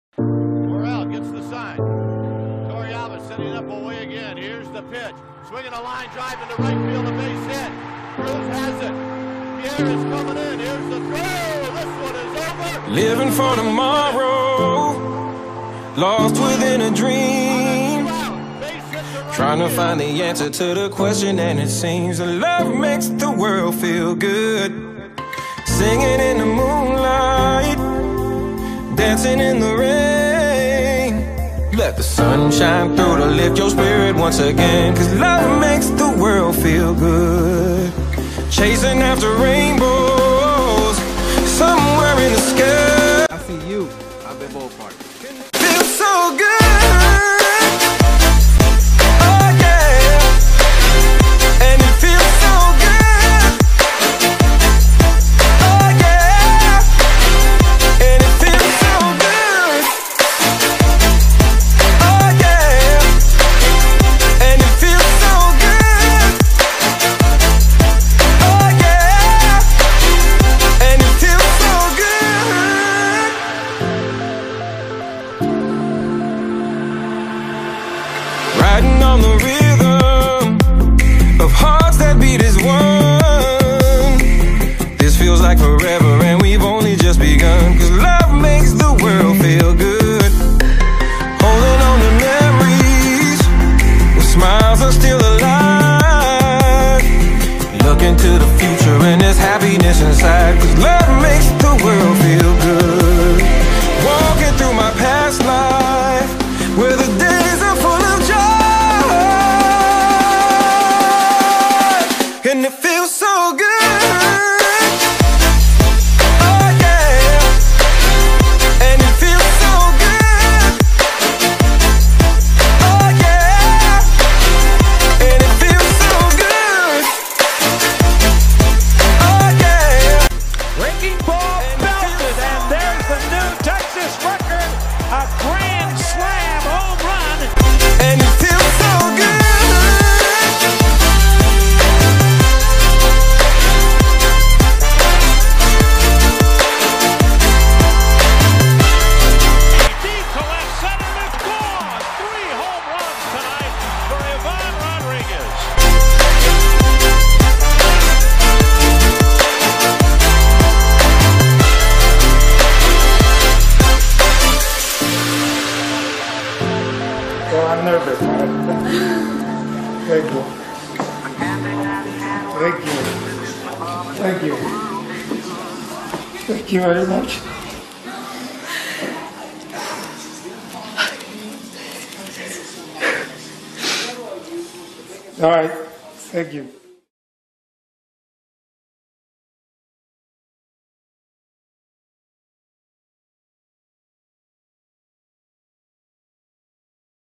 Ivan Rodriguez: Career Highlights
Some notable highlights of Ivan Rodriguez's 20-year career in Major League Baseball, leading to an eventual first-ballot induction into the Baseball Hall of Fame.

Moments:
0:00 - Walk-off single in extras (2003 NLDS Game 3)
0:46 - Walk-off home run
0:56 - First career HR
1:03 - Throwing out Ken Griffey Jr.
1:11 - Celebrating after Kenny Rogers' Perfect Game
1:18 - Famous Nolan Ryan Fight
1:24 - Throwing out 3 baserunners in 1 game
1:37 - Throwing out Lind trying to steal 3rd
1:50 - Game-tying home run in the 9th off of Mariano Rivera
2:03 - 9 RBI game (Franchise record)
2:17 - 3 home run game against the Twins
2:36 - Holding on to ball after a collision at the plate
2:41 - 300th career home run
2:47 - Reaction to news of getting into the Hall of Fame